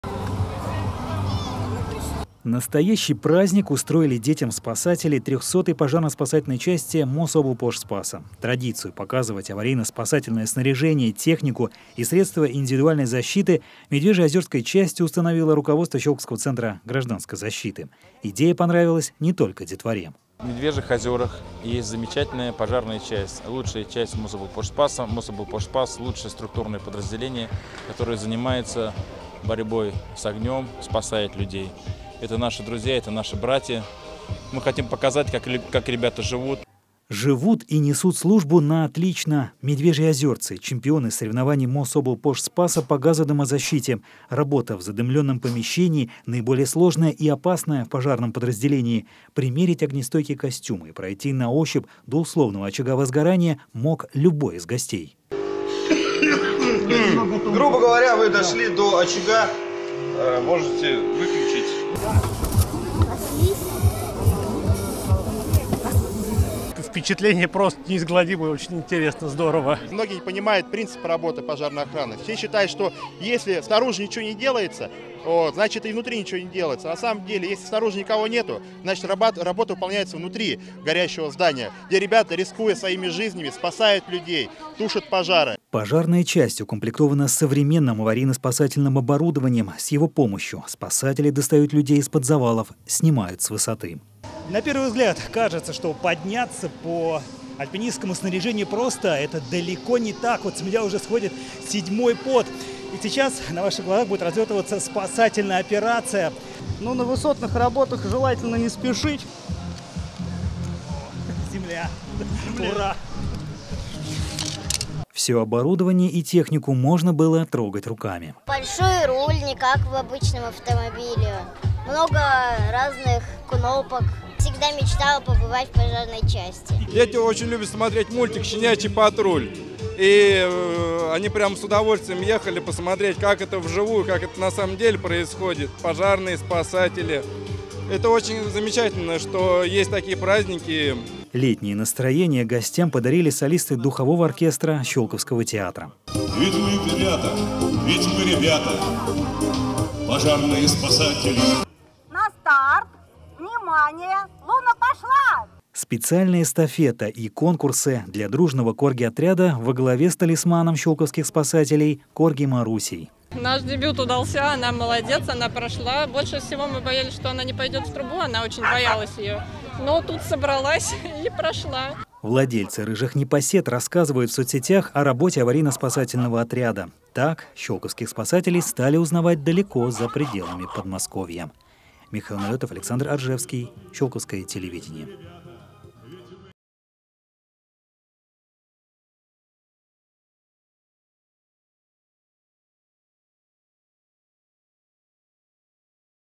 Ежегодный день открытых дверей состоялся в 300-й пожарно-спасательной части «Мособлпожспаса»
Прикоснуться к профессии. Ежегодный день открытых дверей состоялся в 300-й пожарно-спасательной части «Мособлпожспаса». Каким арсеналом технических средств располагают пожарные и спасатели щёлковского Центра гражданской защиты, узнала наша съёмочная группа.